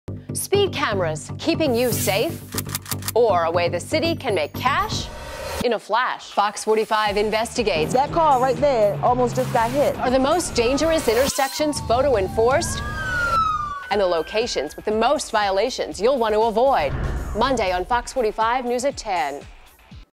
SPEED CAMERAS
THE SPEED CAMERAS THAT CATCH THE MOST OFFENDERS.
AND... AN INVESTIGATION INTO OUR MOST DANGEROUS INTERSECTIONS.
MONDAY...ONLY ON FOX 45 NEWS AT TEN.